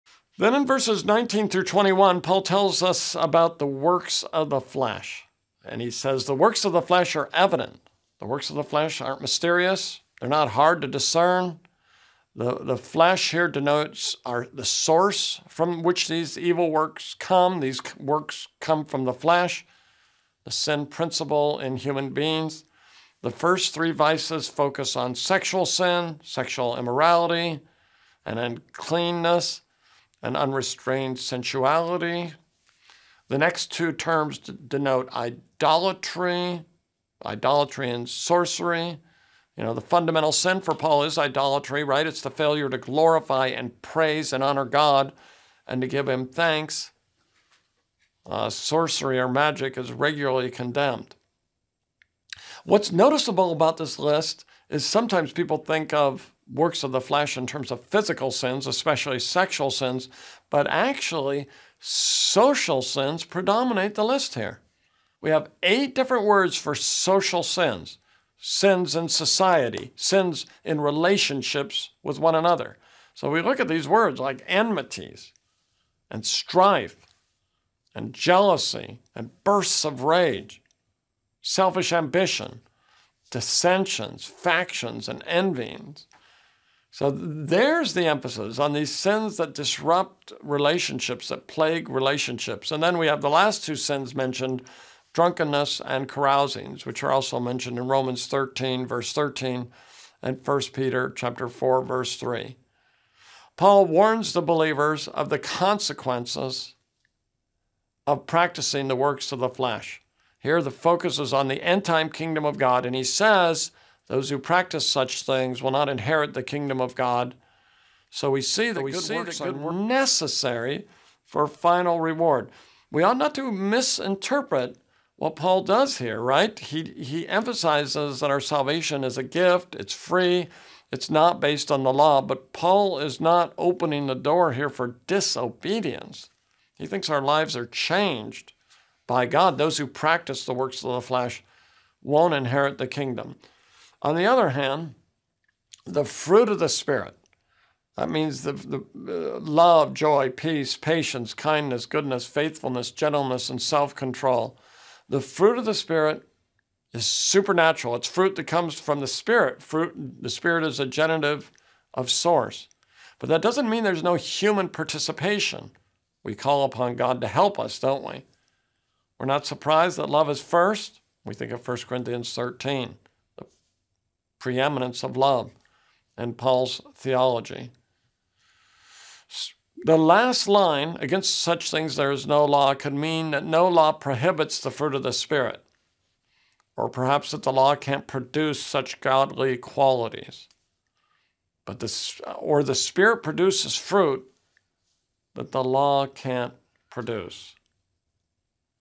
What Are the Fruit of the Spirit and the Works of the Flesh
A truly Spirit-empowered believer is going to be obvious in their fruit. They'll demonstrate what Pauls calls "the fruits of the Spirit." He also points out that the "works of the flesh" are fairly apparent too."

In his online course on Galatians, Thomas R. Schreiner teaches us about walking in the Spirit by contrasting these two lists. This video is adapted from Schreiner's course.